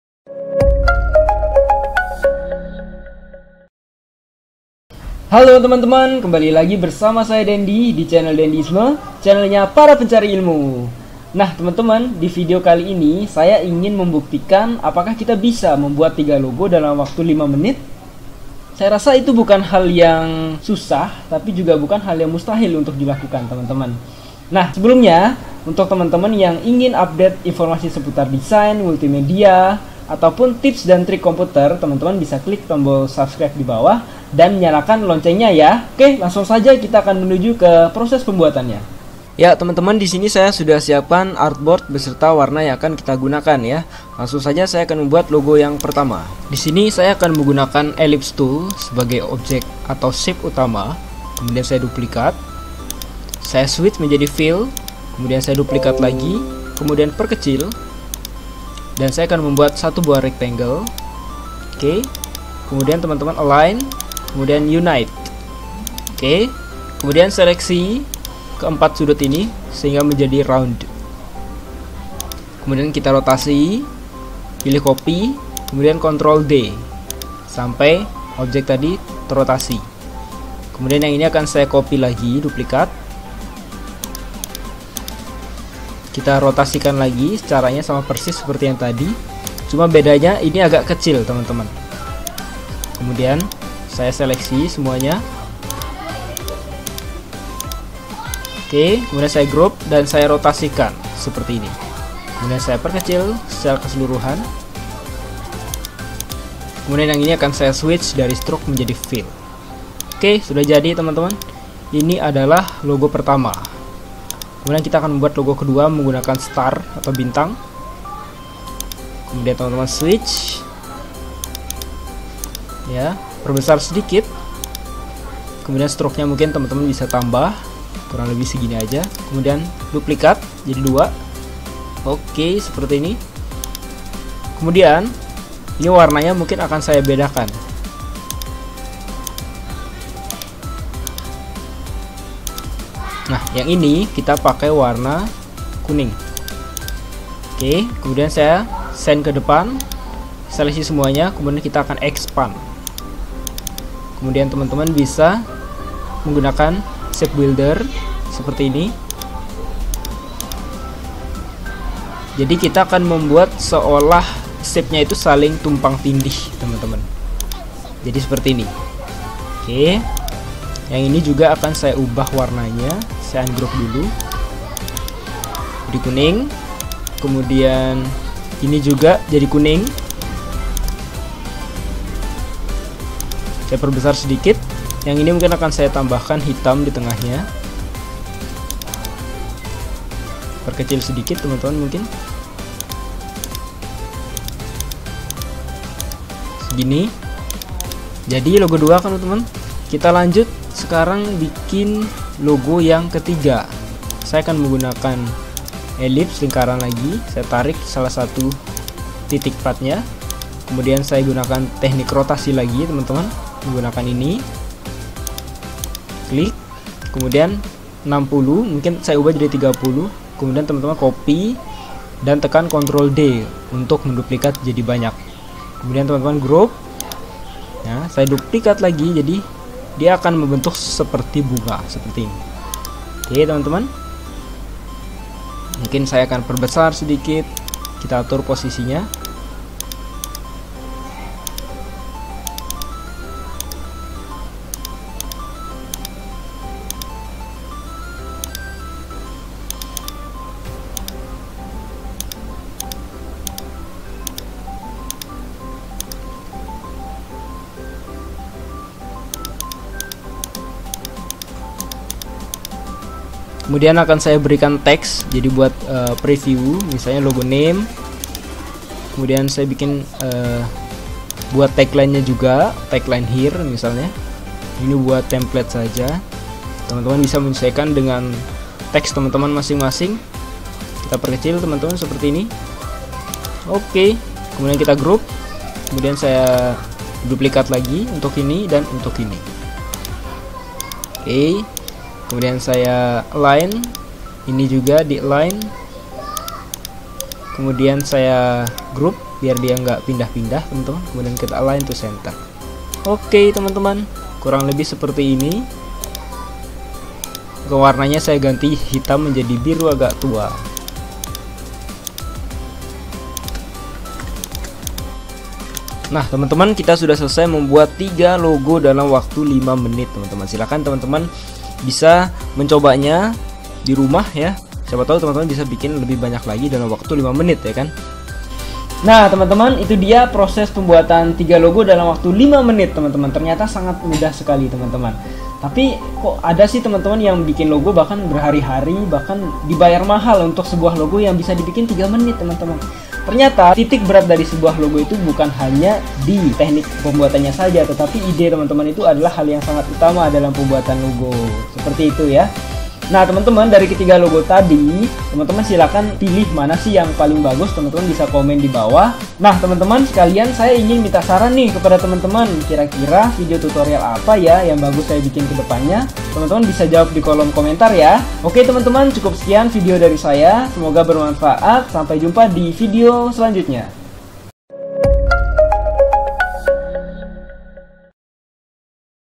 Cara Membuat Logo Dengan Cepat | 5 Menit Bisa Bikin 3 Logo
Cara Membuat Logo Dengan Cepat
5 Menit Bisa Bikin 3 Logo

Video ini berisi penjelasan tentang cara cepat buat logo Menggunakan Adobe Illustrator CC. Materi video ini menjelaskan secara rinci cara mudah desain logo. Pelajari cara belajar desain logo yang keren ini dengan belajar adobe illustrator bahasa indonesia. Anda juga akan mengetahui ciri ciri logo dan belajar desain grafis
Anda Desainer Grafis atau fotografer?
Ingin Mulai Hasilkan Dollar dengan Jualan Desain dan Foto di Situs-situs Microstock?

Terima Kasih.